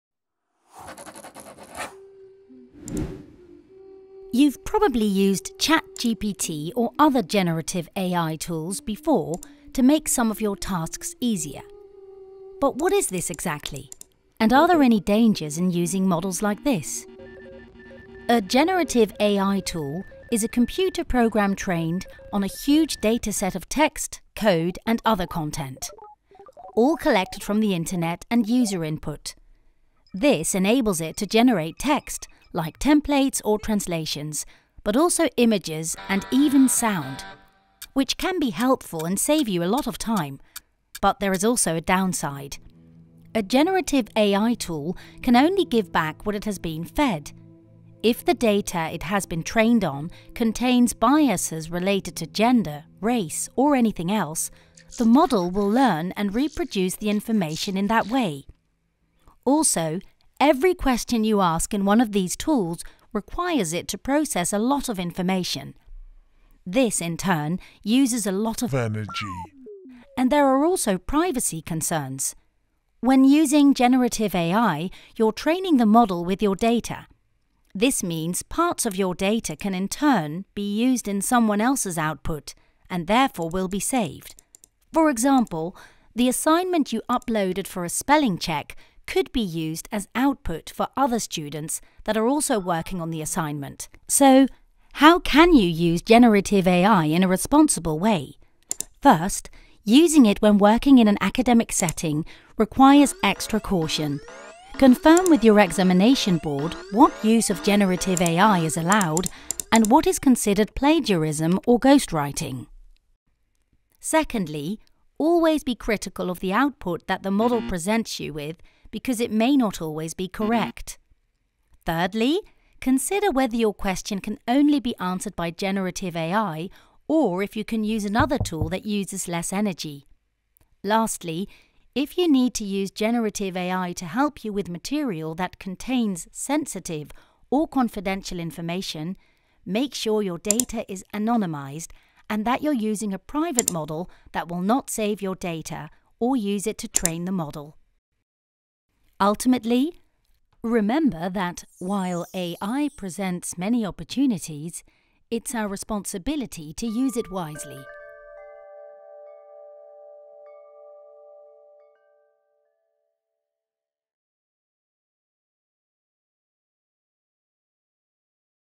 How to use generative AI responsibly?
You've probably used ChatGPT or other generative AI tools before to make some of your tasks easier. But what is this exactly, and are there any dangers in using these tools?

This video was developed by the Community for Learning & Innovation (CLI) in collaboration with EDIS of Erasmus University Rotterdam (EUR).

Animation by Studio Schurk.